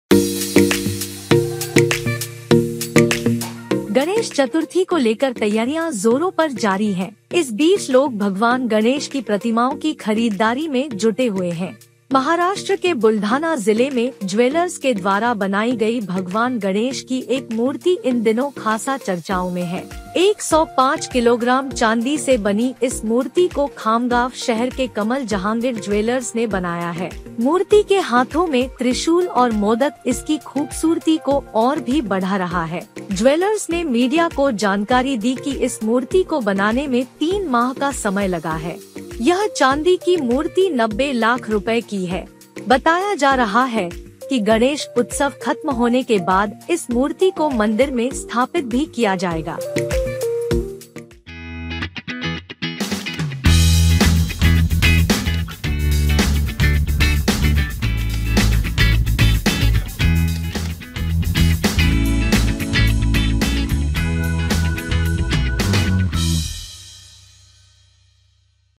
90 लाख में बनकर तैयार हुई 105 किग्रा चांदी की गणेश मूर्ति, दर्शन के बाद नहीं हटेगी आपकी भी नजर
गणेश चतुर्थी को लेकर तैयारियां जोरों पर जारी हैं। इस बीच लोग भगवान गणेश की प्रतिमाओं की खरीददारी में जुटे हुए हैं। महाराष्ट्र के बुलढाना जिले में ज्वेलर्स के द्वारा बनाई गई भगवान गणेश की एक मूर्ति इन दिनों खासा चर्चाओं में है। 105 किलोग्राम चांदी से बनी इस मूर्ति को खामगांव शहर के कमल जहांगिड़ ज्वेलर्स ने बनाया है। मूर्ति के हाथों में त्रिशूल और मोदक इसकी खूबसूरती को और भी बढ़ा रहा है। ज्वेलर्स ने मीडिया को जानकारी दी कि इस मूर्ति को बनाने में 3 माह का समय लगा है। यह चांदी की मूर्ति 90 लाख रुपए की है। बताया जा रहा है कि गणेश उत्सव खत्म होने के बाद इस मूर्ति को मंदिर में स्थापित भी किया जाएगा।